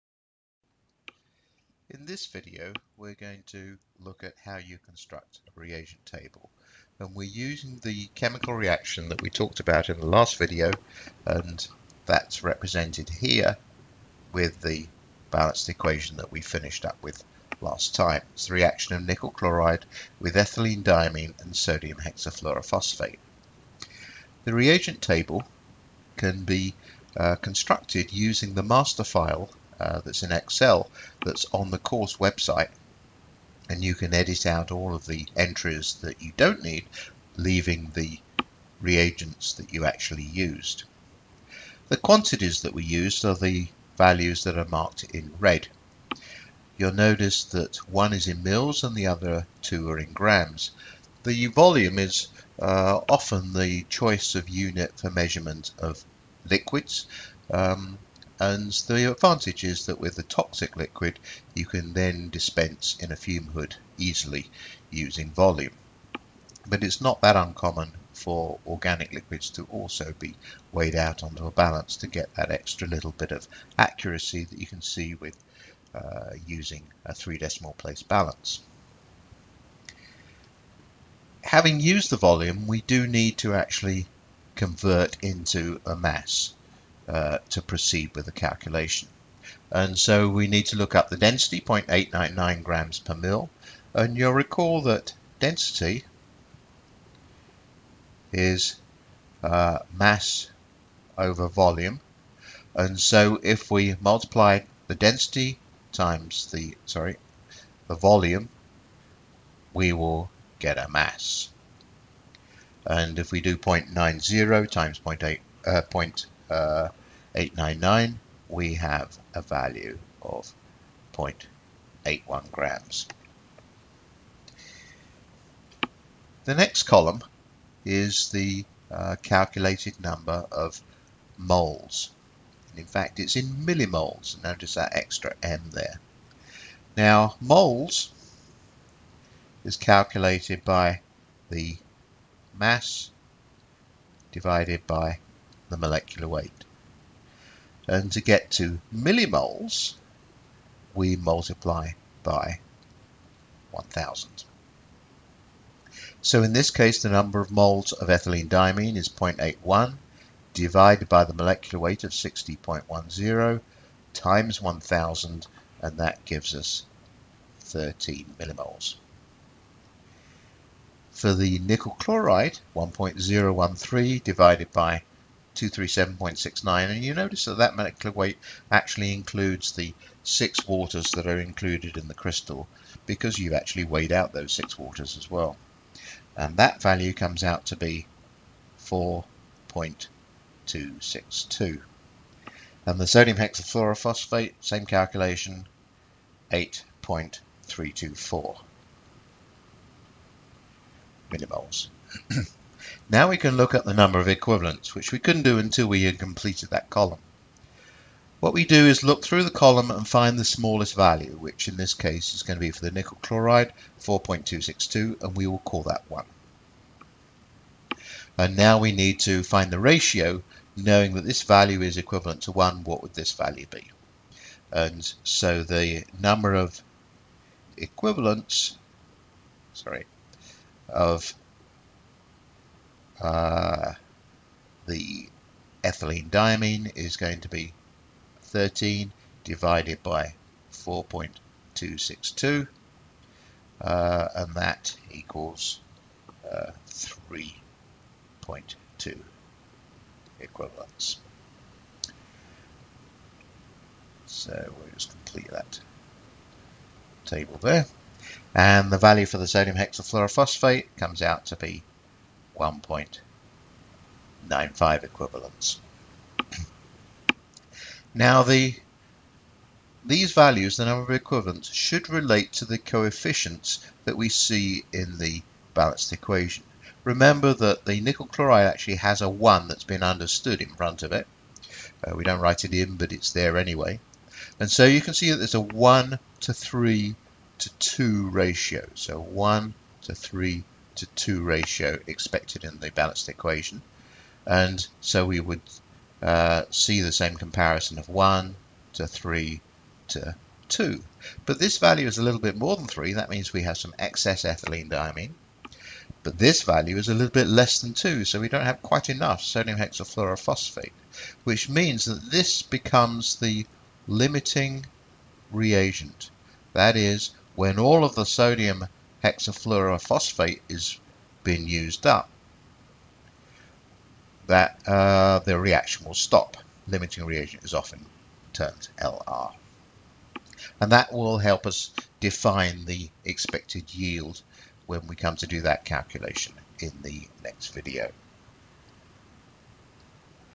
reagent table
A reagents table is created and the entries discussed in terms of their relationship to the stoichiometric equation.